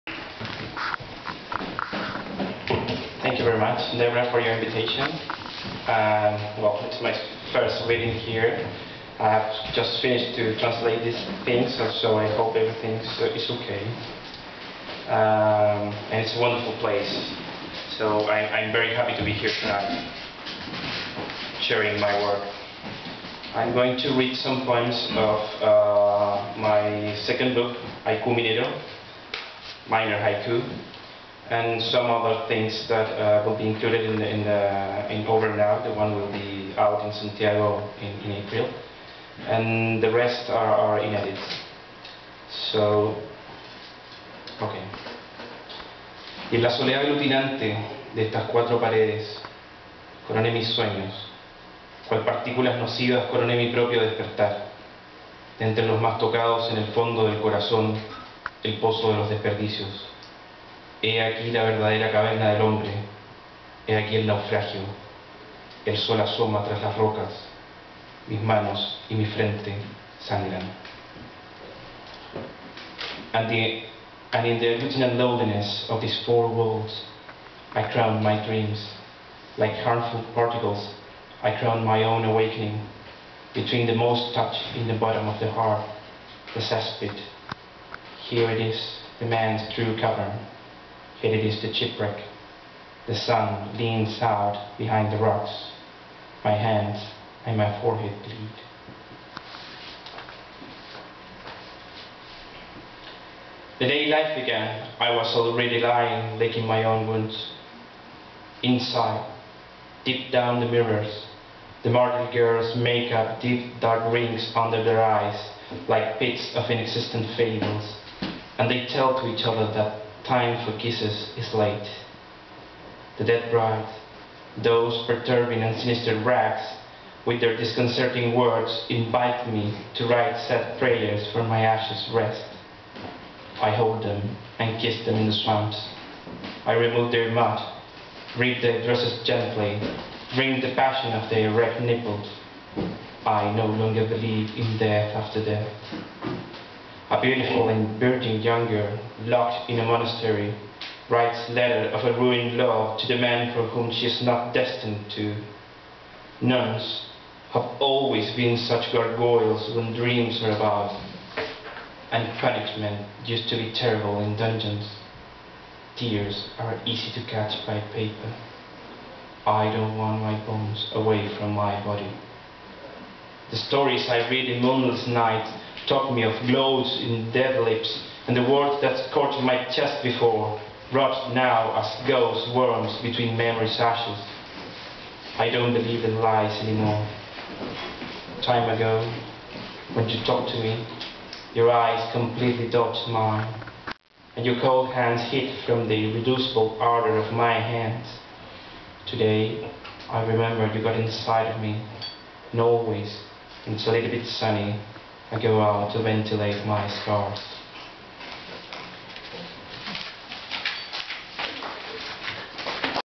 03-28-09 - Carlos Soto-Román - PAPP-UCAL Event - 1 of 4
From the Poetic Art Performance Project's March "Avant Garde" event, hosted by Debrah Morkun at the University City Arts League.